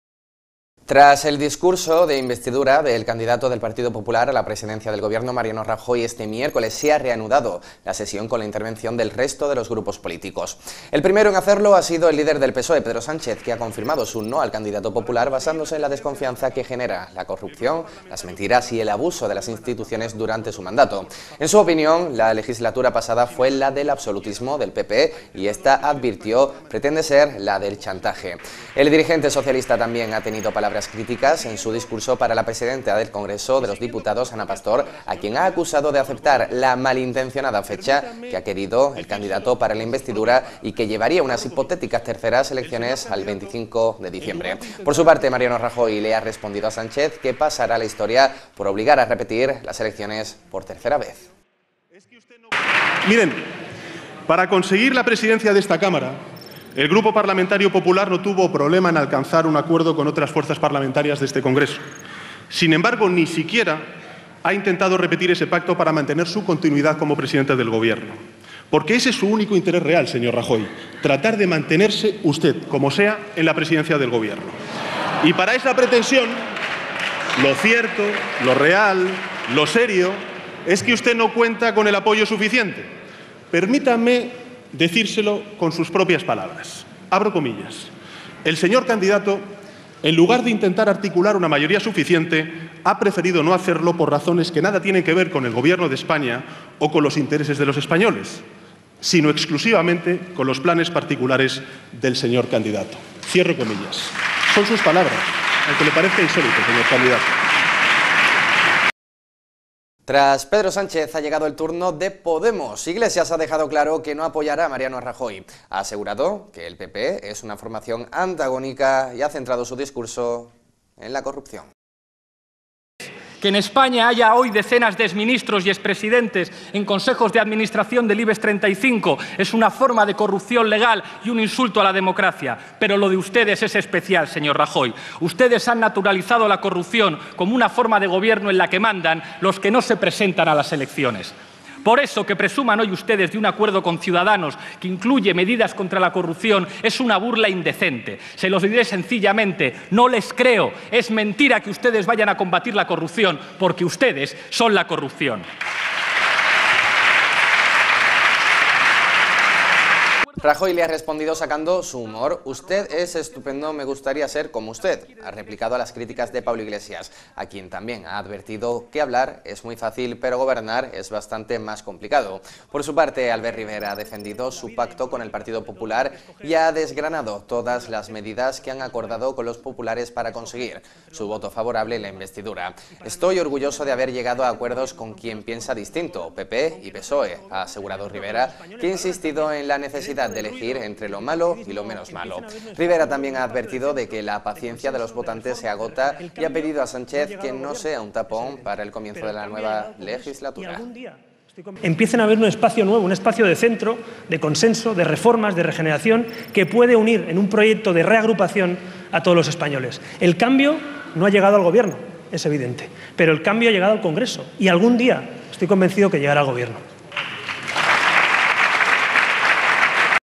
Málaga 24h TV - Réplicas de Sánchez, Iglesias y Rivera al discurso de Investidura
Tras el discurso de investidura del candidato del PP a la presidencia del Gobierno, Mariano Rajoy, este miércoles se ha reanudado la sesión con la intervención del resto de los grupos políticos. El primero en hacerlo ha sido el líder del PSOE, Pedro Sánchez, que ha confirmado su no al candidato popular basándose en la desconfianza que generan la corrupción, las mentiras y el abuso de las instituciones durante su mandato.  En su opinión, la Legislatura pasada fue la del "absolutismo" del PP y ésta, advirtió, pretende ser la del "chantaje".

El dirigente socialista también ha tenido palabras críticas en su discurso para la presidenta del Congreso de los Diputados, Ana Pastor, a quien ha acusado de aceptar la "malintencionada" fecha que ha querido el candidato para la investidura y que llevaría unas hipotéticas terceras elecciones al 25 de diciembre.

Por su parte, Mariano Rajoy le ha respondido a Sánchez que pasará a la historia por obligar a repetir las elecciones por tercera vez.

Tras Pedro Sánchez, ha llegado el turno de Podemos. Iglesias ha dejado claro que no apoyará a Mariano Rajoy, de cuyo partido ha asegurado que su formación es antagónica, y ha centrado su discurso en la corrupción del Partido Popular.

Por su parte, Albert Rivera ha defendido su pacto con el Partido Popular, y ha desgranado todas las medidas que han acordado con los populares para conseguir su voto favorable en la investidura. “Estoy orgulloso de haber llegado a acuerdos con quien piensa distinto, PP y PSOE”, ha asegurado Rivera, que ha insistido en la necesidad de elegir entre “lo malo y lo menos malo”.

English: After the inaugural speech of the PP candidate for president of the Government, Mariano Rajoy, it has resumed Wednesday session with the participation of other political groups. The first to do so has been the leader of the PSOE, Pedro Sanchez, who has not confirmed the popular candidate based on mistrust generated by corruption, lies and abuse of institutions during his tenure. In his opinion, the last Legislature was that of "absolutism" of the PP and this, he said, aims to be the "blackmail".

The socialist leader has also had critical words in his speech for the president of the Congress of Deputies, Ana Pastor, who has been accused of accepting the "malicious" date wanted the candidate for investiture and take a third hypothetical elections December 25th.

Meanwhile, Mariano Rajoy has responded to Sanchez that will make history by forcing repeat elections for the third time.

After Pedro Sanchez, it is now the turn of Podemos. Iglesias has made clear it will not support Mariano Rajoy, whose party has ensured that training is antagonistic, and has focused his speech on corruption Popular Party.

Meanwhile, Albert Rivera has defended his pact with the Popular Party, and has shelled all measures agreed with popular for its favorable vote on the investiture. "I am proud to have reached agreements with those who think differently, PP and PSOE", said Rivera, who has insisted on the need to choose between "bad and less bad".